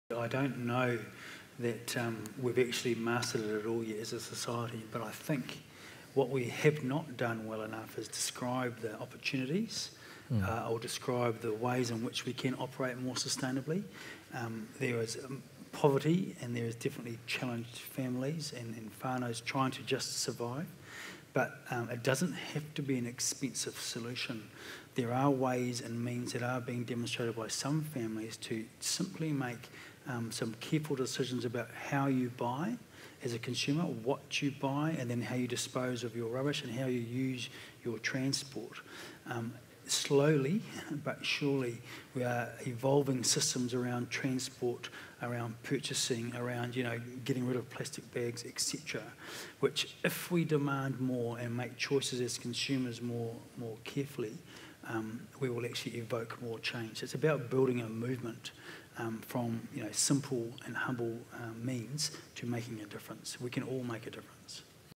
Opportunities of Sustainability - Jan Hania - Te Tauihu Talks
Jan Hania shares his ideas on the opportunities of sustainability as part of the Te Tauihu Intergenerational Strategy series.